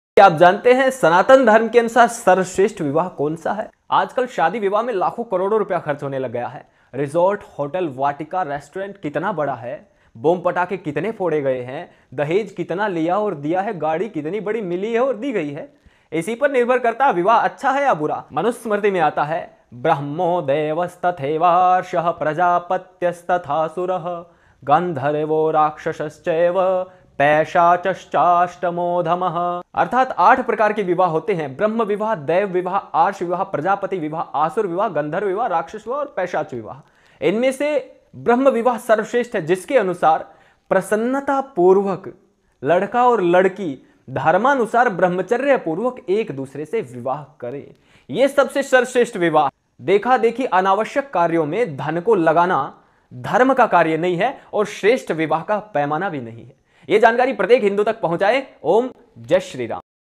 Which is the Best MARRIAGE in Hinduism? 🔥 Rahul Arya Shorts
Vedic Vivah Shadi kaise karte hai? Kaun sa vivah uttam aur sarvshreshth hai? Manusamriti me ek shlok aata hai jisme sanatan dharam ke anusar marriage ka sahi tarika bataya gya hai.